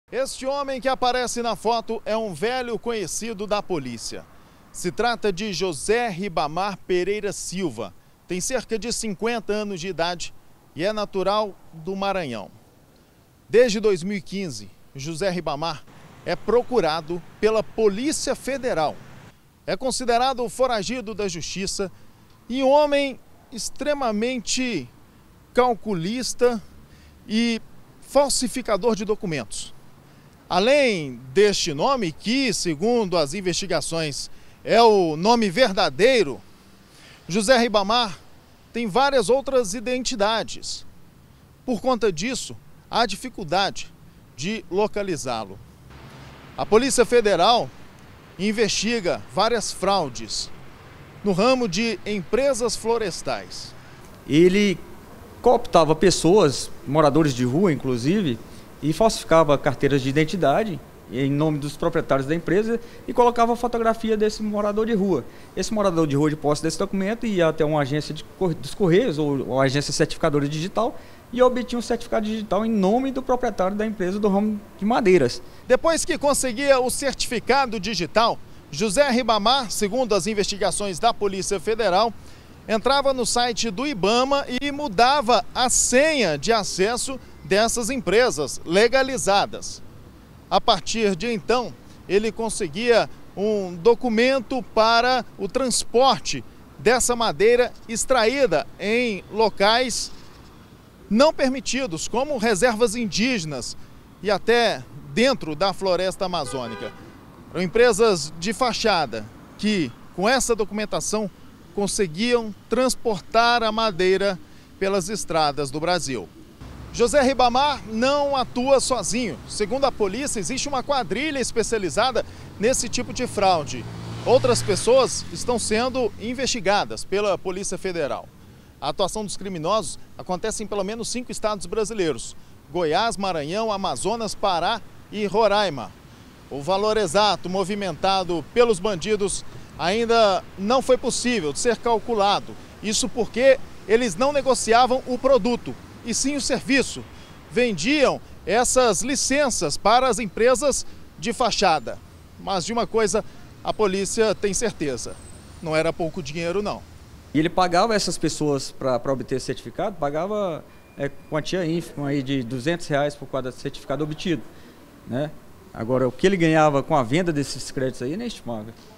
BG - Golpe pela internet: polícia federal procura estelionatário - 15-08-2018
A polícia federal está a procura de um homem suspeito de estelionato. Ele usava pessoas humildes para falsificar documentos e roubar créditos ambientais pela internet.